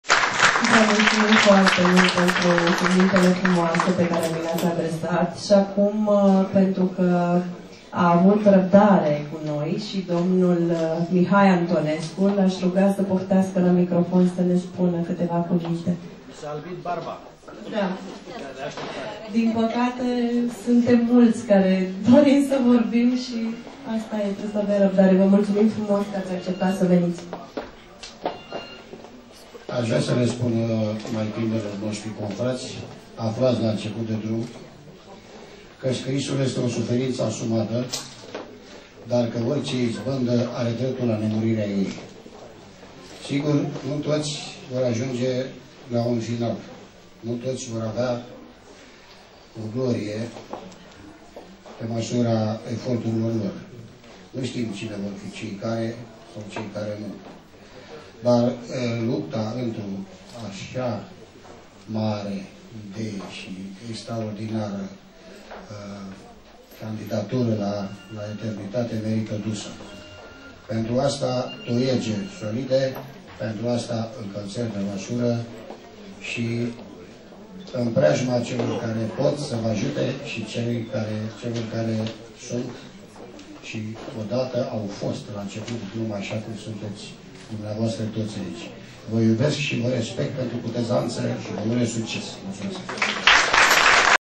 Dl Mihai Antonescu despre "Antologia fara titlu"
22 aprilie 2016; Casa de Cultura Calderon Bucuresti, scriitoru/criticul Mihai Antonescu vorbeste despre creatiile (poezi&proza) elevilor (intre 8-12 ani) Trupei de Teartu JOC; Cartae: "Antologia fara titlu", coordonata de actrita Magda Bacescu, aparuta la Editura Betta, editor Nicolae Rosu; Filmare: Antoaneta Rădoi (prozatoare, cameraman/fotoreporter)